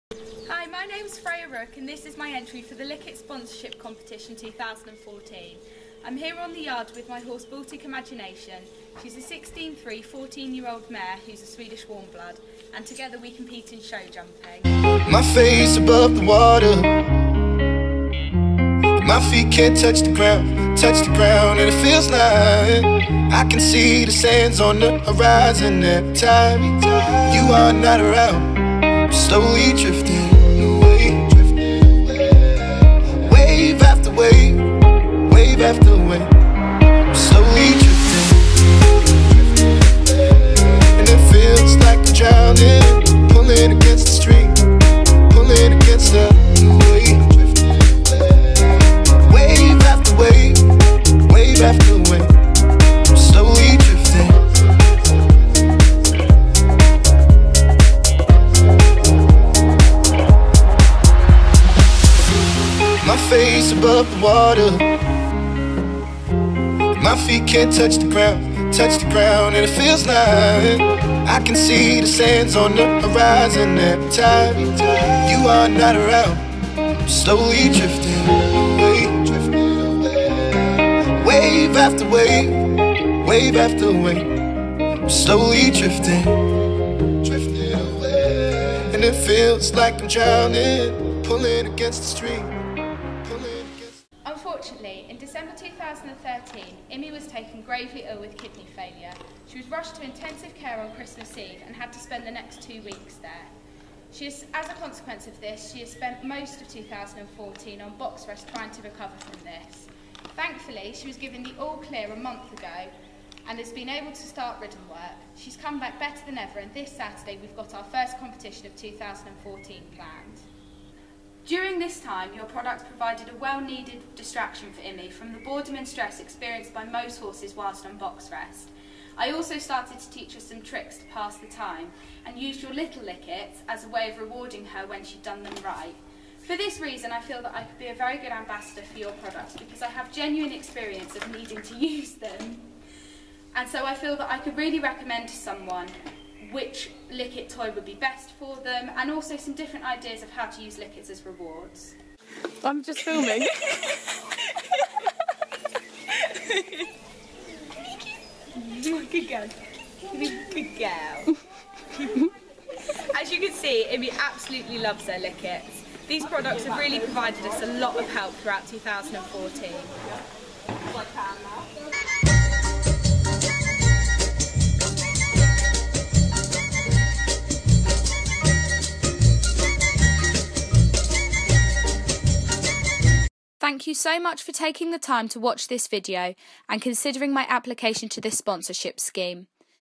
Likits Sponsorship Video 2014
Freya Rooke and Baltic Imagination application for Likits sponsorship programme 2014

regular updates are posted onto all of these links over the years i have owned her we have built a up a very loyal following of immi's antics besides she's such a character whats not to love?